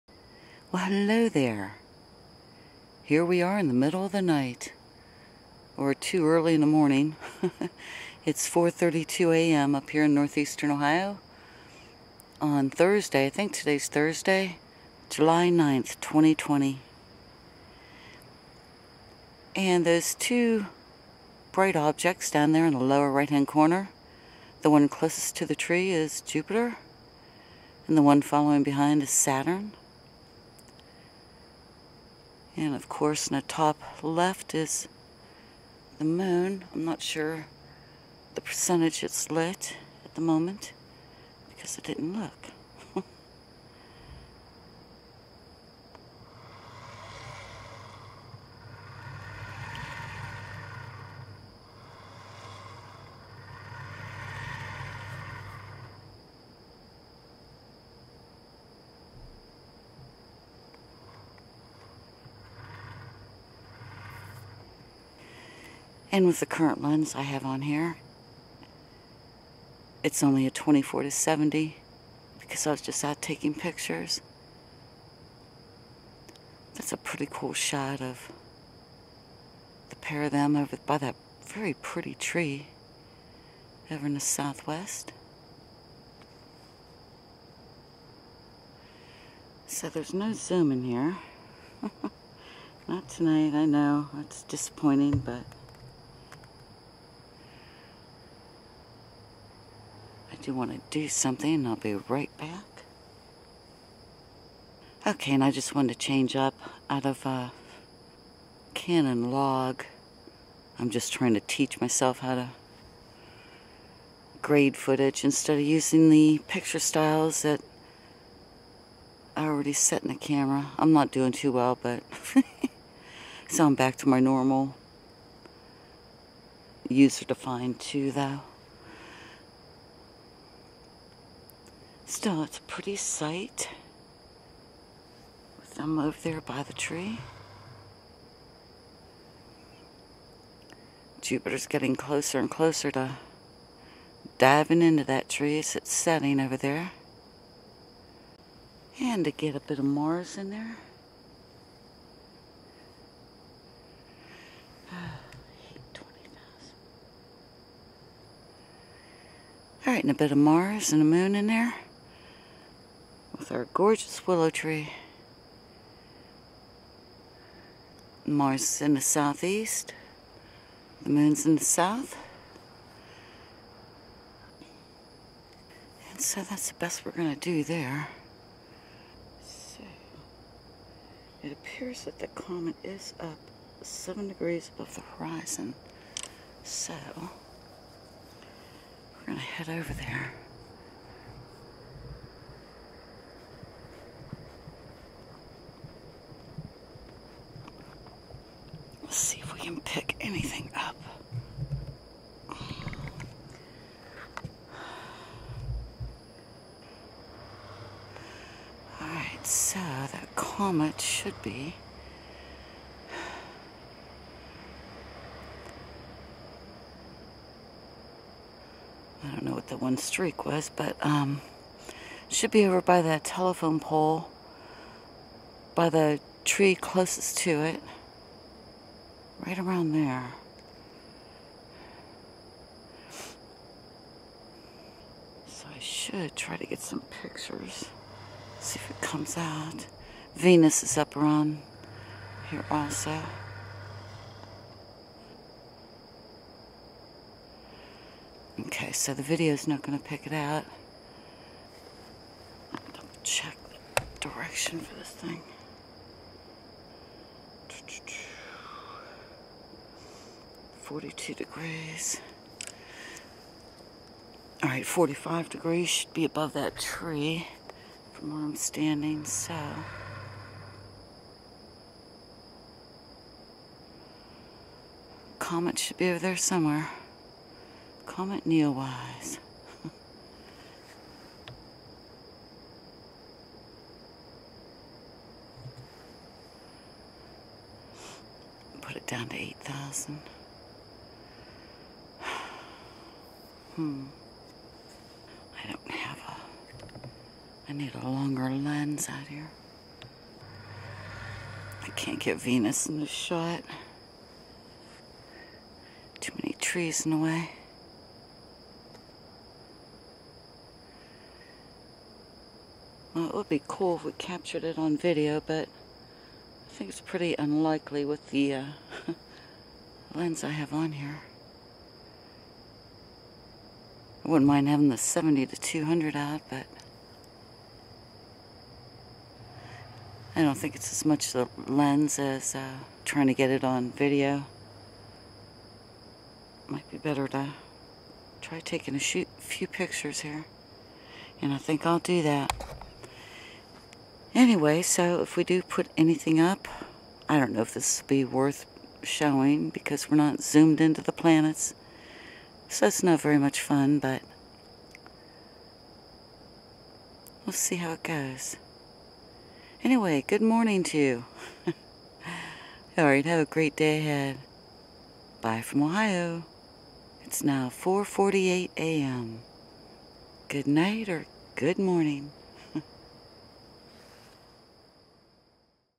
Comet Neowise Search, Saturn, Jupiter, Mars, Moon82% - Wide Angle Footage Canon R Camera
*No luck on capturing the Comet.*
Each morning the comet will be heading to the left into a mass of trees so it's doubtful I'll have a chance to catch it here before sunrise. Possibly in a week or so I may have a chance in the evening when it'll be over in the NW sky after the sun goes down. Around Tuesday July 14th, 2020, the Comet will begin to rise in the NW after sunset, so each day thereafter it'll get higher in the sky and move from right to left, from the NW to the West. Good viewing to you. :)

1st part with Saturn, Jupiter & the Moon are filmed in Canon Log on the Canon R camera & Tamron 24-70mm F/2.8 lens, (while I try to learn how to grade log footage), and Mars plus the search for Comet Neowise are filmed without log. Apologies for no zoomed footage as I know it’s not as much fun, but this was the only gear I had taken out.

4:17am to 4:48am, Thursday July 9, 2020, NE Ohio (USA), South, SW, SE & Eastern Sky, 70°F (21°C)

Filmed on the Canon EOS R Full Frame Mirrorless Camera & Tamron 24-70mm Lens
Recorded onto the Atomos Ninja Flame 7” Monitor/Recorders in 4k
Photos edited in Lightroom Classic

Here's the equipment used in this video:

-Canon EOS R Mirrorless Full Frame Camera
-Tamron SP 24-70mm F/2.8 Lens
-Atomos Ninja Flame 7" 4K Field Monitor/Recorder

-Tascam DR-100 MK III Audio Recorder